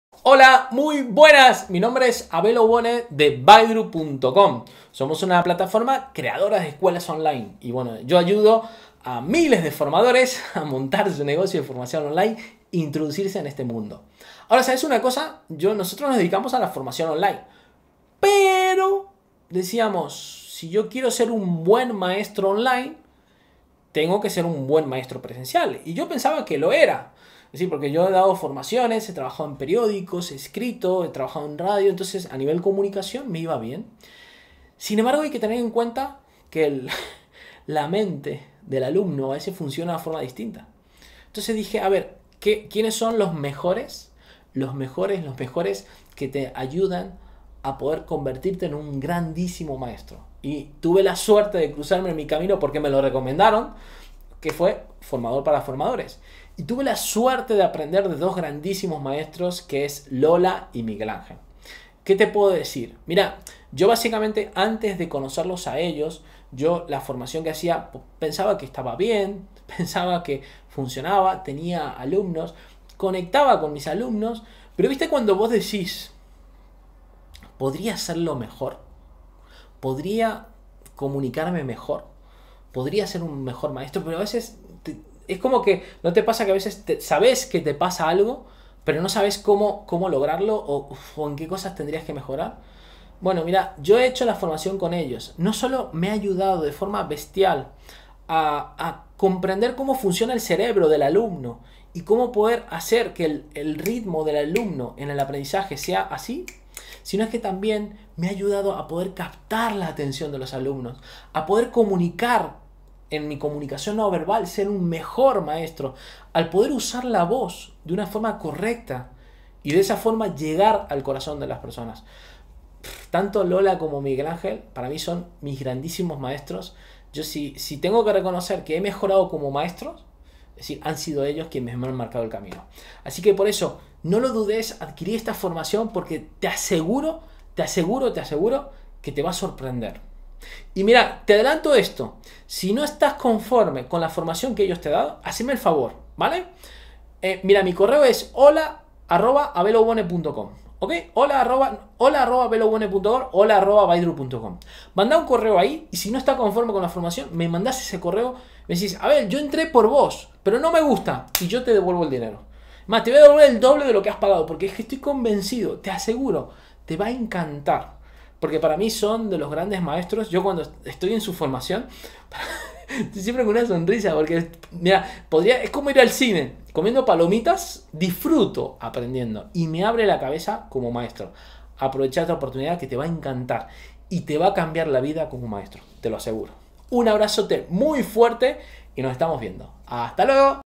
Testimonio Abel Aubone-Vidroop
Abel Aubone es para nosotros el referente en formación online, la ha llevado a otra dimensión, no en vano con el nos estamos formando como formadores online. Sin embargo, lo conocimos mientras é a pesar de dedicarse a la formación online decidió participar en todos nuestros talleres para dominar también la formación presencial.